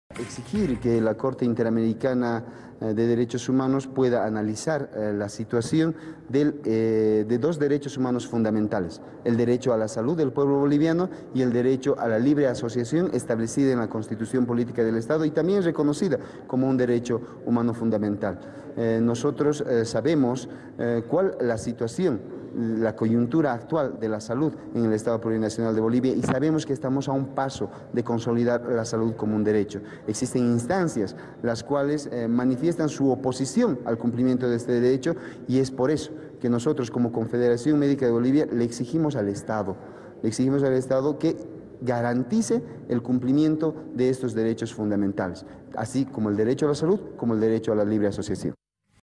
Confederación de Médicos exigen ante CIDH se garantice el derecho a la salud y la libre afiliación
La Confederación de Médicos denunció ante la Comisión Interamericana de Derechos Humanos (CIDH) que en Bolivia hay pequeños sectores los cuales se oponen a la aplicación del Sistema Único de Salud (SUS) y la libre afiliación de los galenos.

Jerson Auza, presidente de la Confederación de Médicos, dijo: “Existen instancias las cuales manifiestan su oposición al cumplimiento de ambos derechos. Como Confederación exigimos al Estado y a la CIDH que garanticen el cumplimiento de estos derechos.”